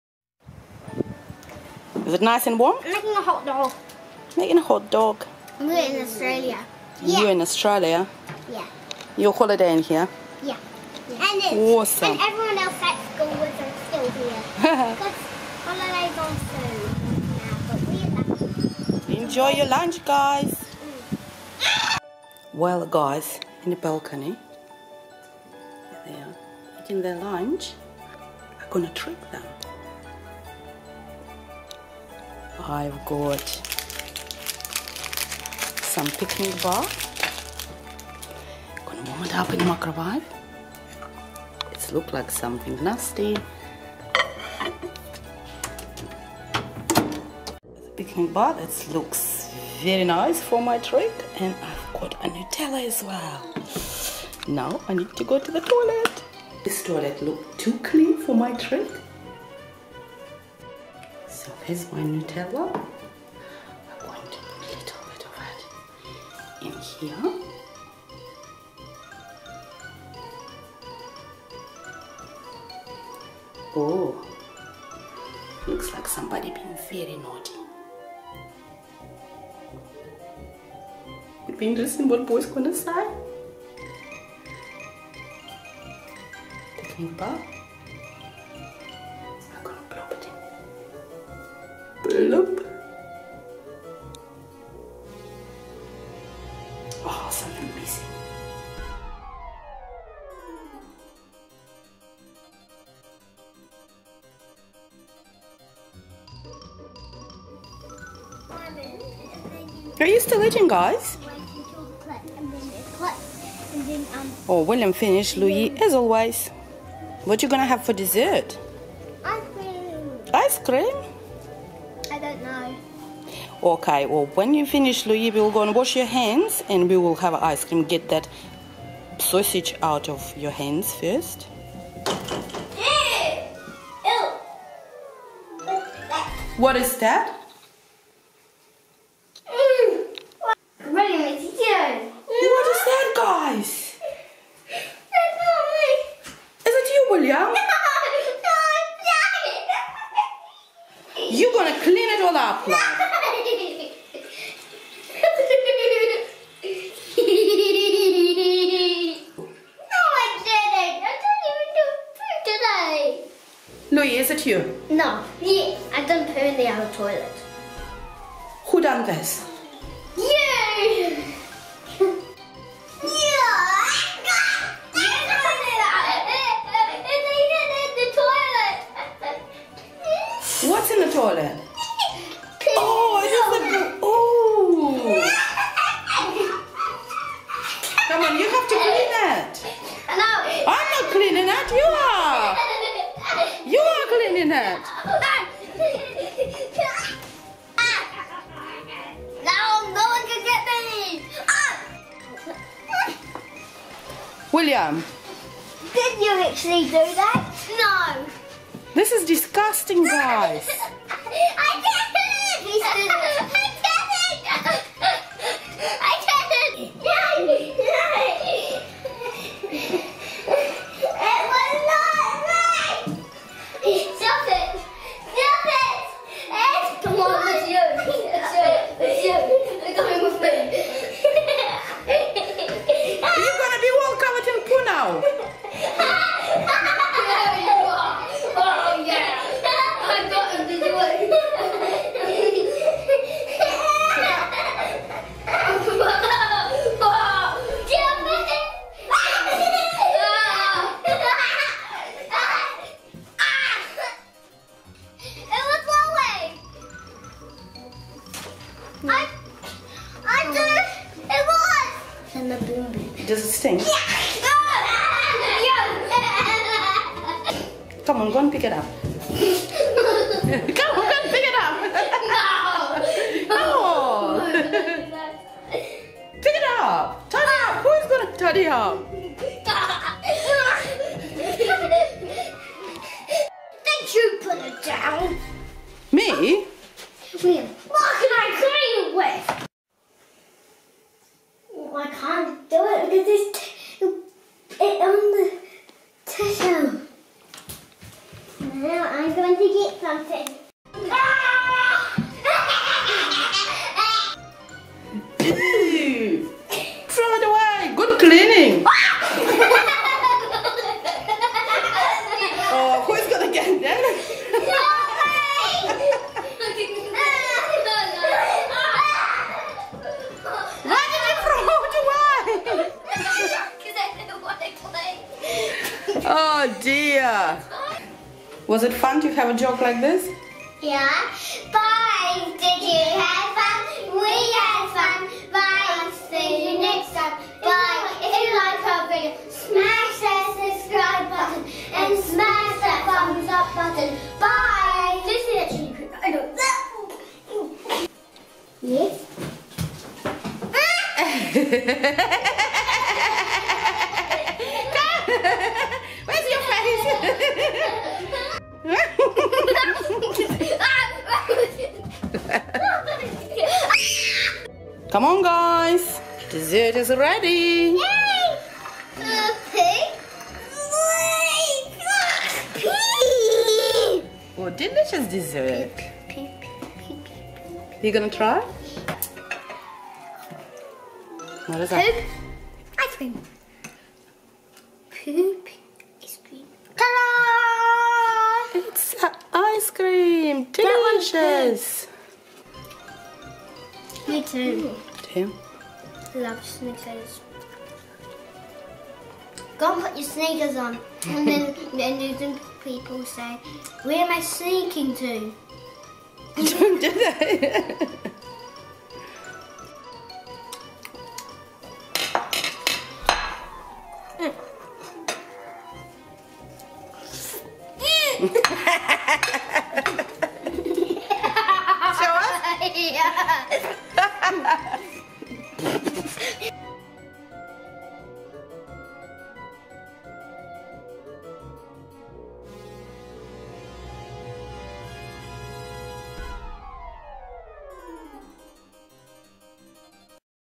Challenge it????? Mommy makes prank on kids!!! Mommy vs. kids!!!!!!
Please Give this video a 👍 👍👍👍   if you loved it!!
Can you prank kids...??? I did and it was awesome fun prank on kids! mommy vs.kids. and the winner of the fun challenge is. ? KIDS AND FUN !